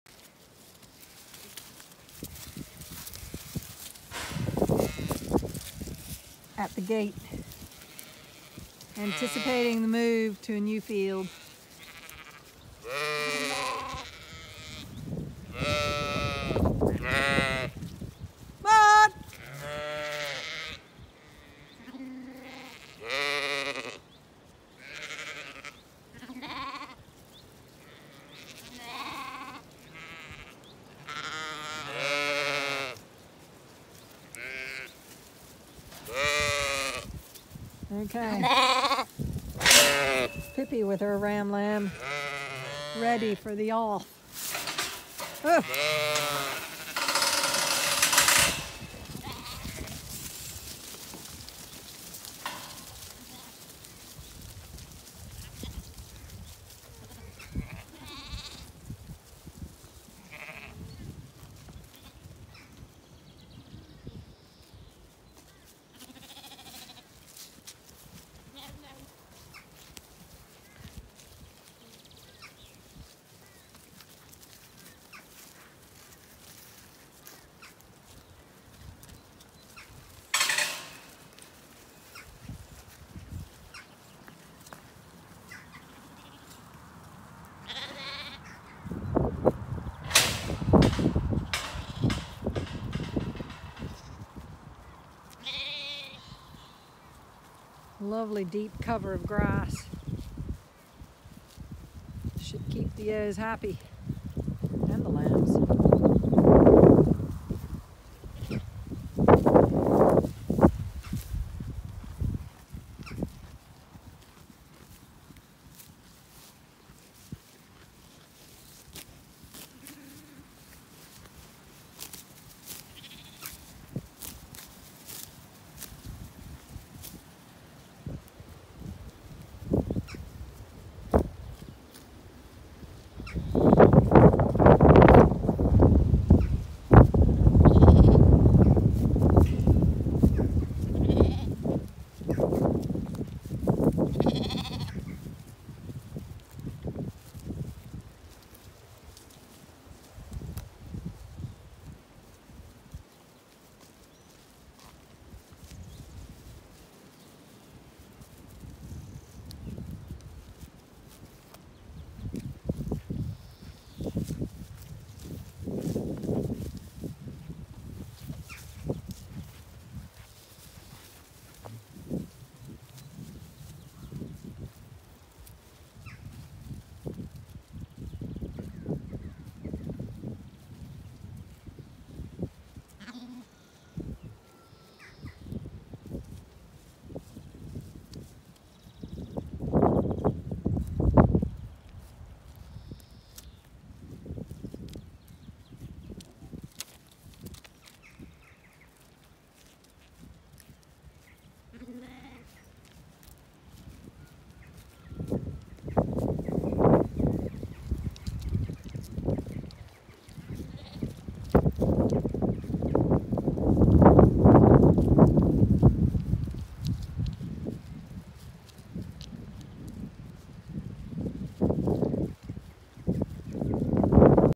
Alpaca, ewes & Lambs move to fresh pasture.
A short walk to fresh grass and very happy sheep. This field has rested for 5 months.